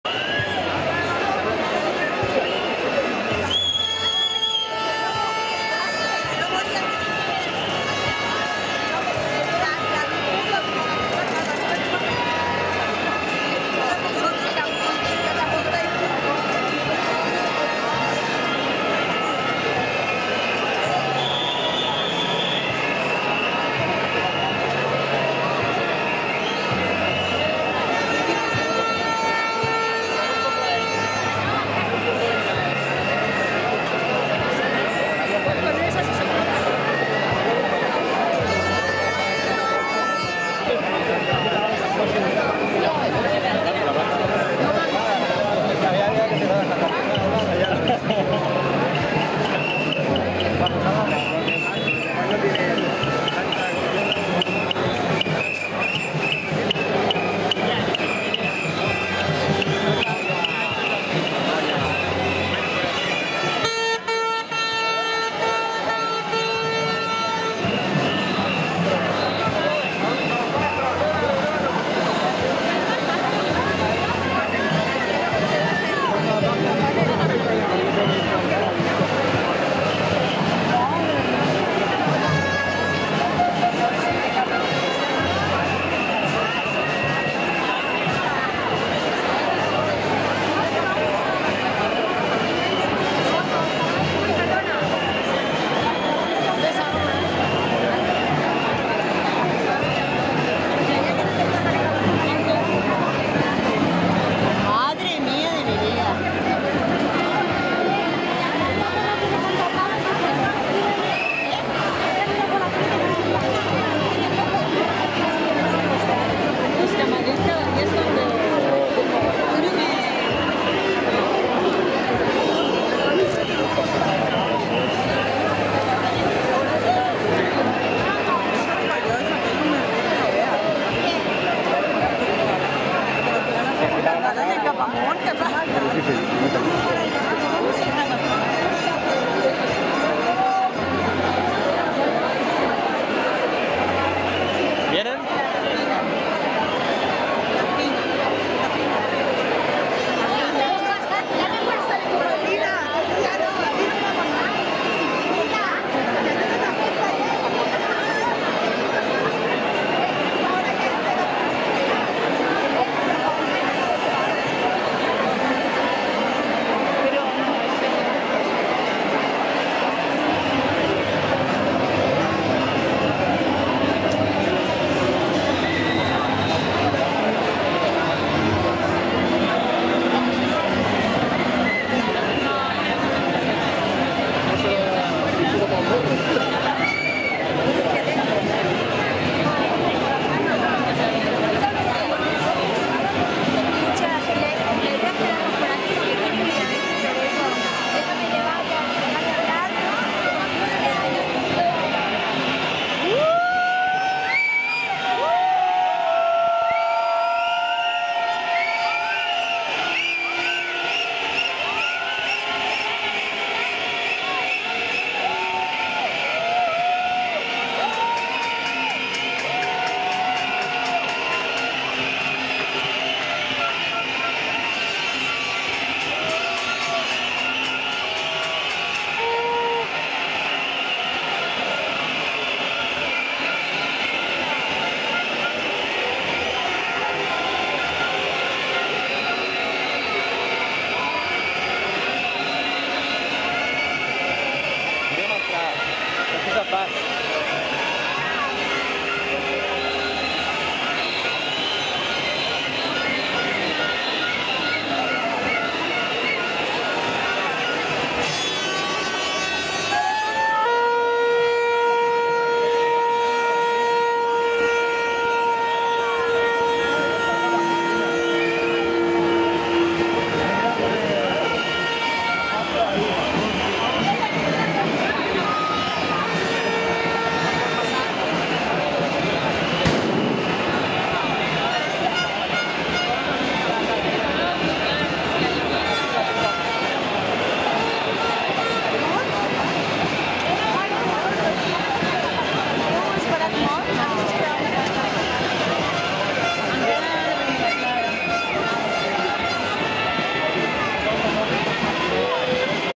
MANI BARCELONA 19/07/2012 - 1a part
primera part de la mani contra les retallades i la corrupció general

(que se jodan ellos)